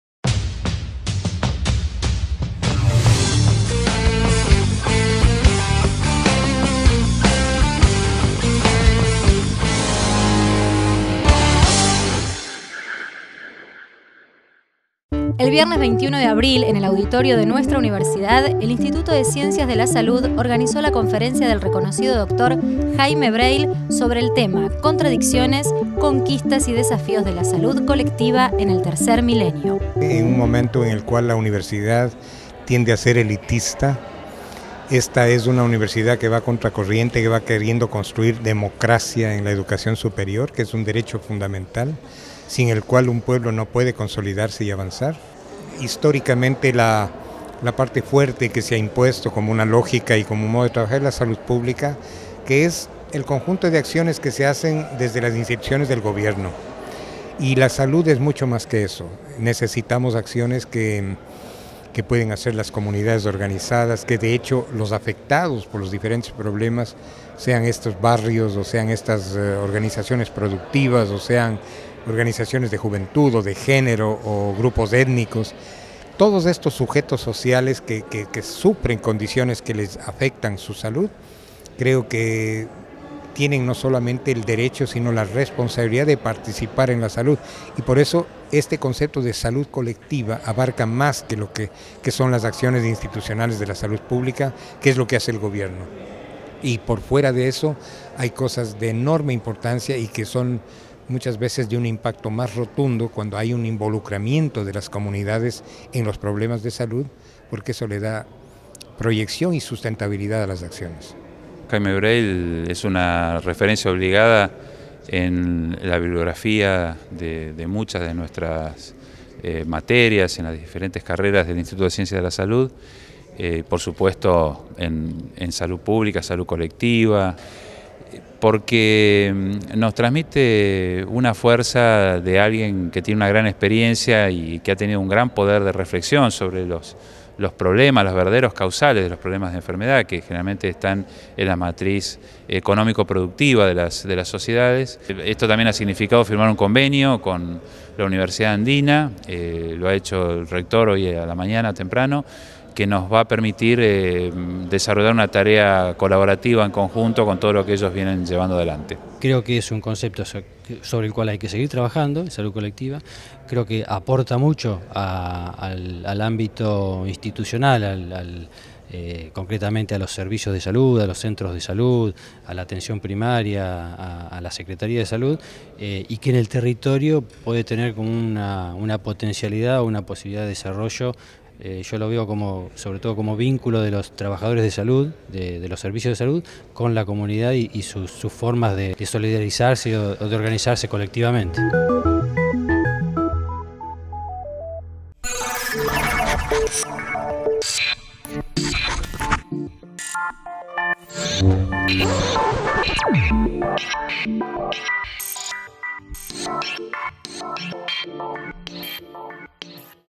Jaime Breilh en la UNAJ.
El viernes 21 de abril en el Auditorio de nuestra universidad, el Instituto de Ciencias de la Salud organizó la conferencia del reconocido Doctor Jaime Breihl sobre el tema: Contradicciones, conquistas y desafíos de la salud colectiva en el tercer milenio.